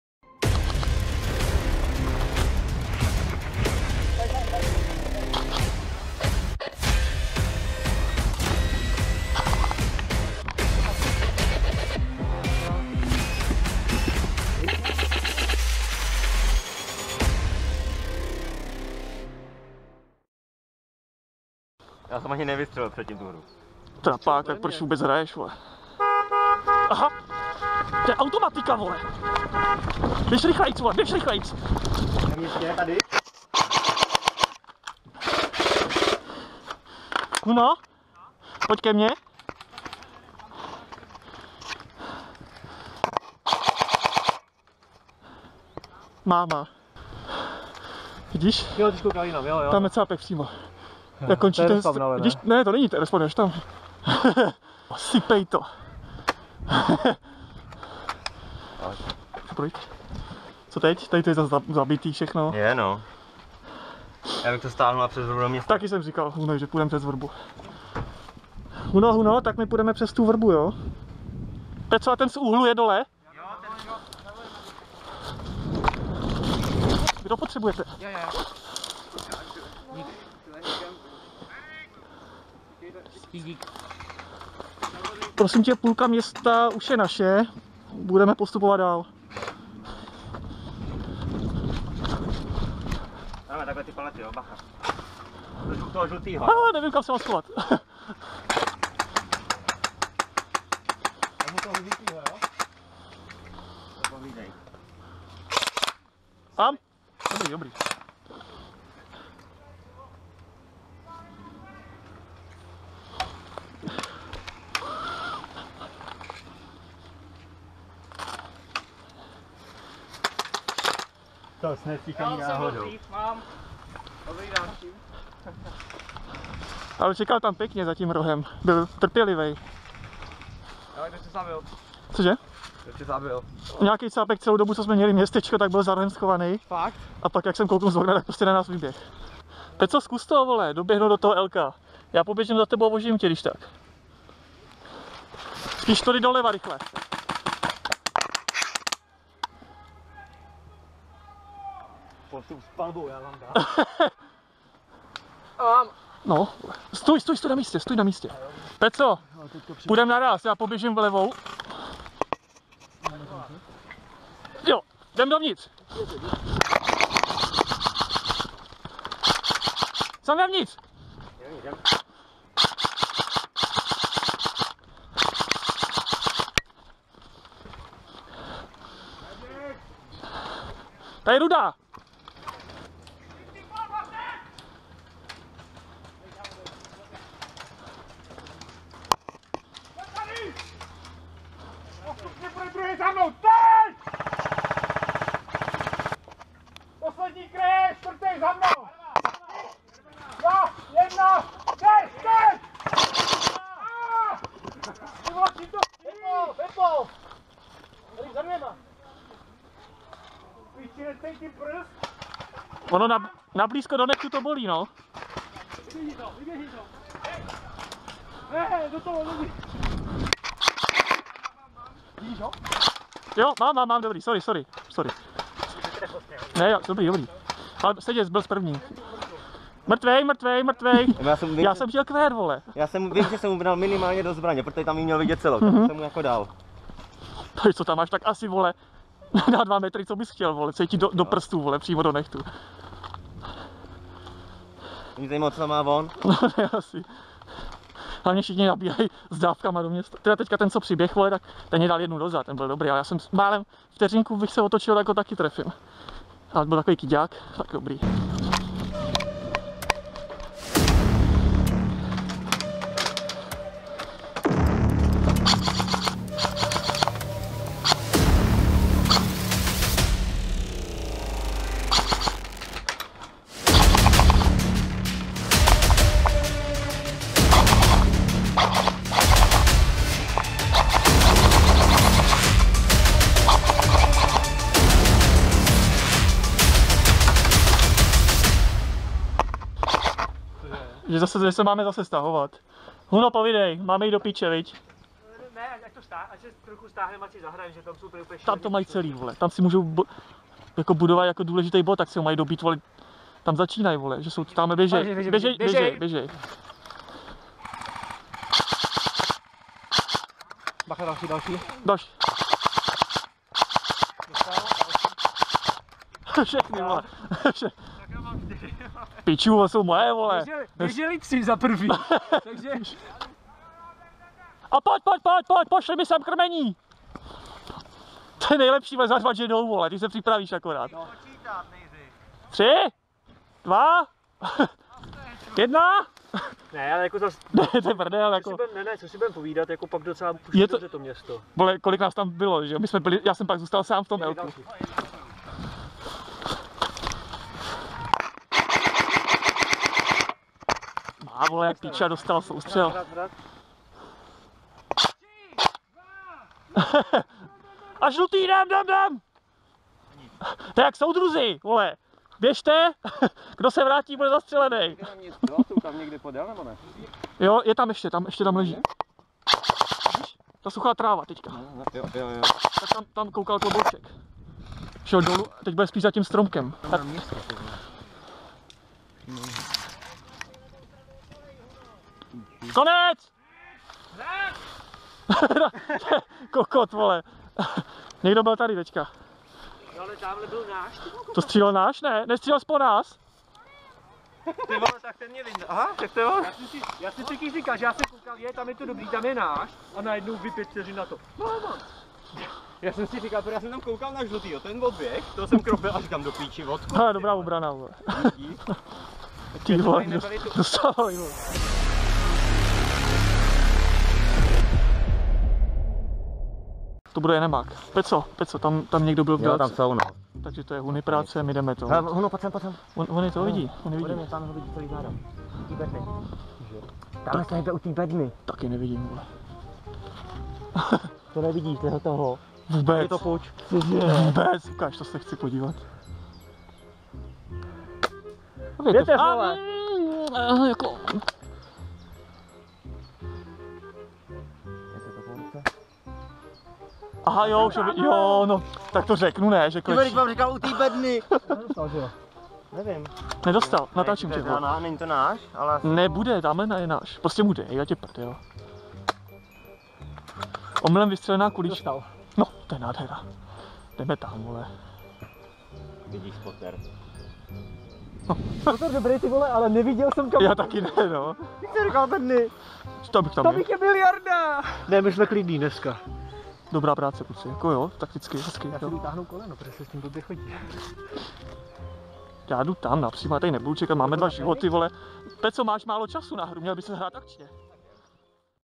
Czech Airsoft [ARENA FENIX] Mad Trio Rampage_Part I
Sestřih z druhé hry na aréně fénix. Zkráceně: dařilo se nám (červeným) slušně ničit vše na co jsme narazili. Protihráč měl velkou motivaci v podobě hráče který odpočítával každý jejich pohyb. Z počátku se to zdálo "nebezpečné" Ovšem po čase se ukázalo, že jen vyzrazuje kdy přijde útok, kde se nacházejí, a tak byl čas se na vše připravit a zamířit :D